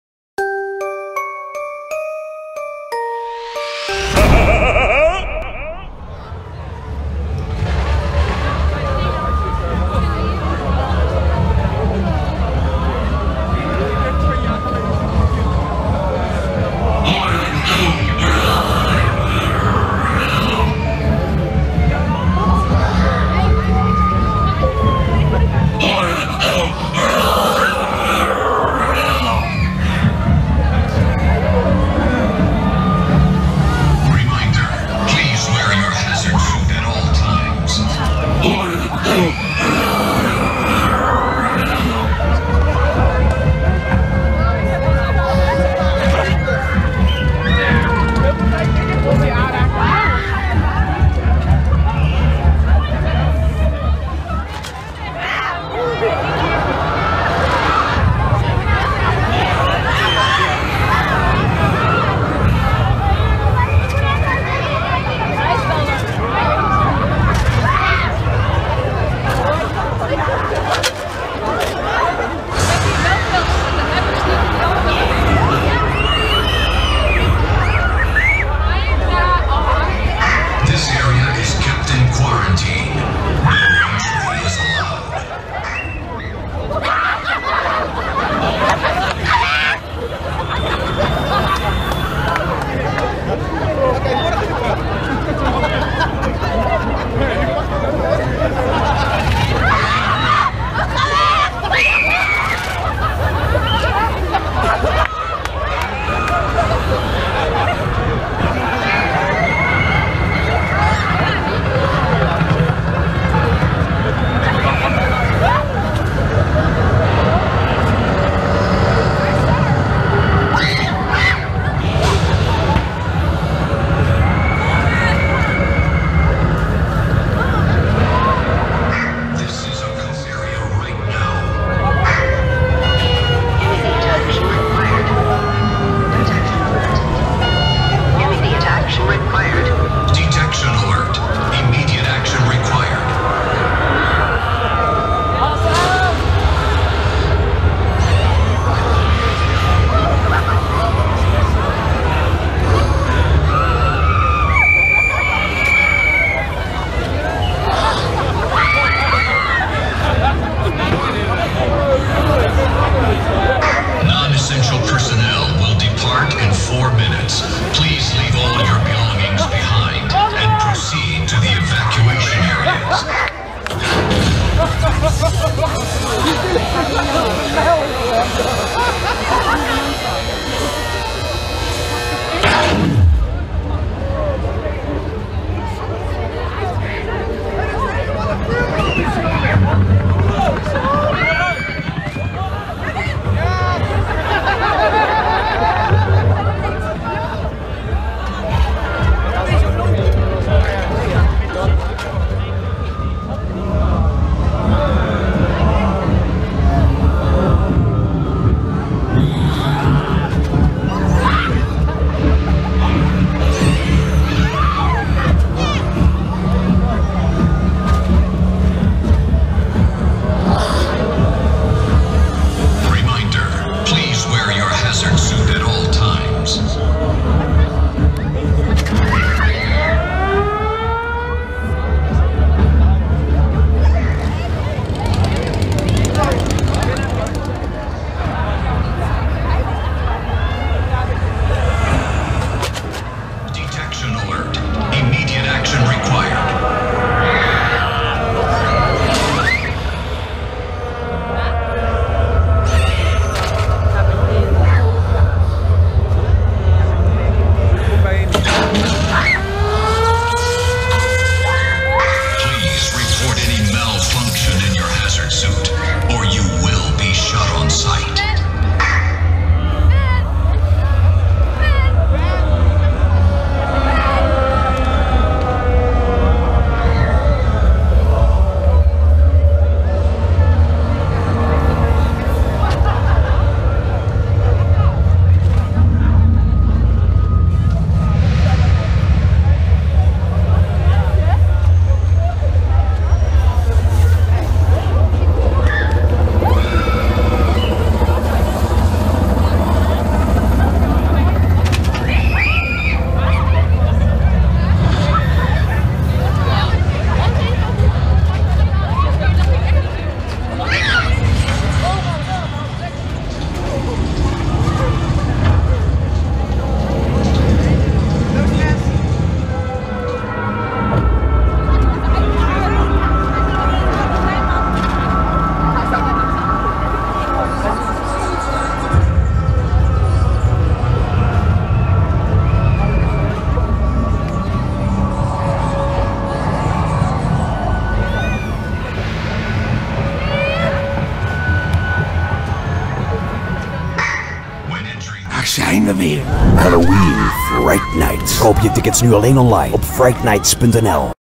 Halloween Fright Nights 2017 - Quarantine (Vrijdag 20-10-2017)
Tijdens Halloween Fright Nights verzamelen rottende zombies, duivelse demonen en bloederige Rednecks zich in Walibi Holland. Met 7 intense Scare Zones, 6 bloedstollende Haunted Houses, een heftige walktrough en een vette horrorshow wordt oktober gruwelijker dan ooit.
Evil is here…